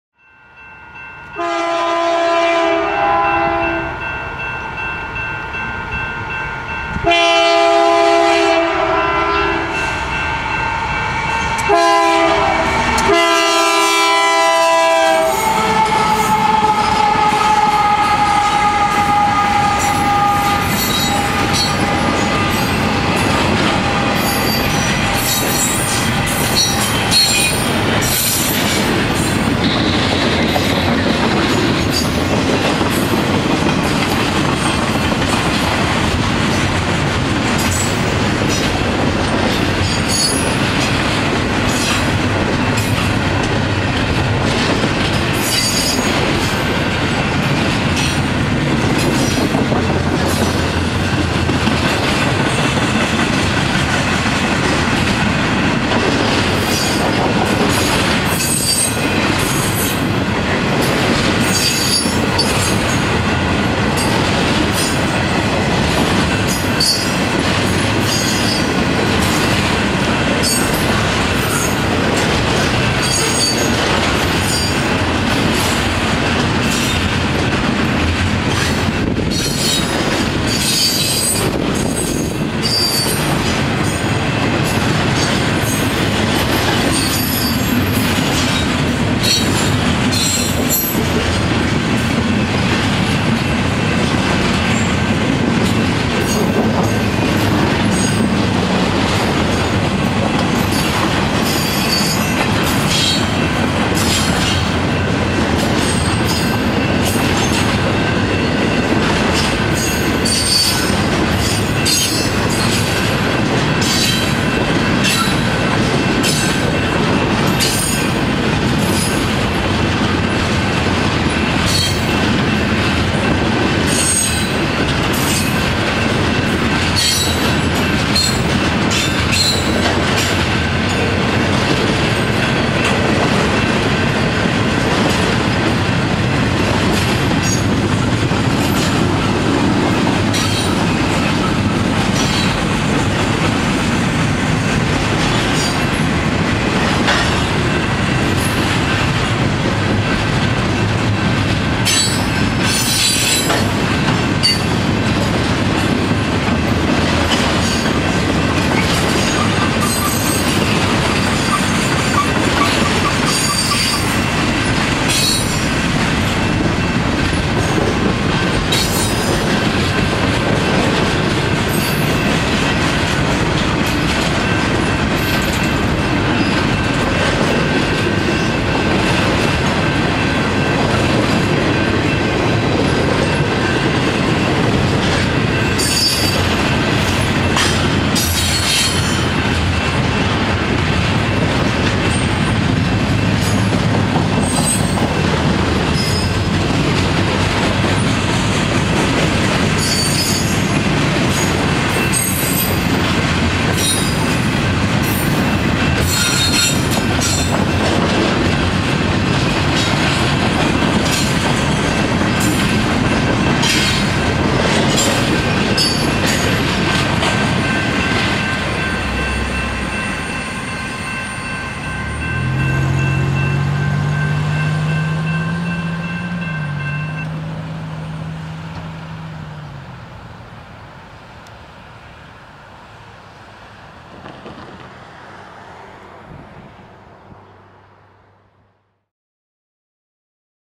CEFX & CP Intermodal EBD 16Jun13
CP Intermodal train EBD with a CEFX locomotive in the lead. Locomotives CEFX 1031 AC44CW and CP 9640 AC44CW. Recorded in Taylor, MI.